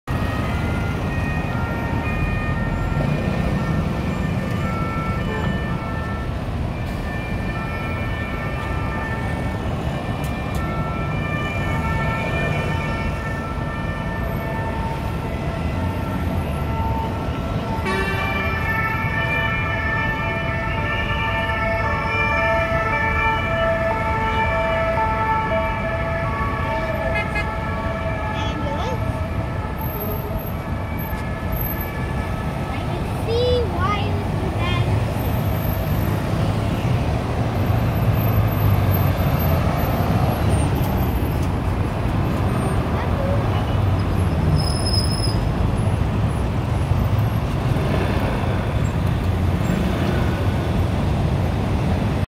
The craziness of Paris - Arc de Triomphe round about
Think we'll pass on driving in Paris!!